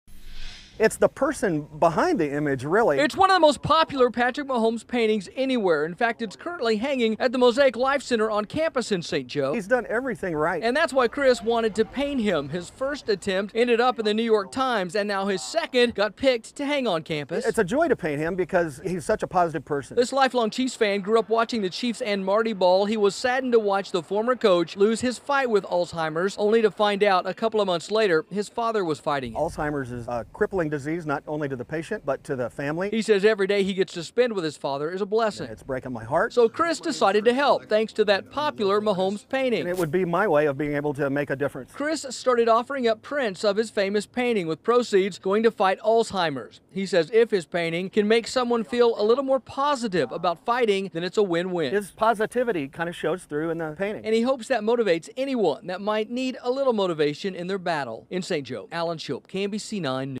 St. Joseph artist using Patrick Mahomes painting to raise money to battle Alzheimer's disease
An artist in St. Joseph is using a painting of Patrick Mahomes as a way to raise awareness for Alzheimer's and to help fight the disease.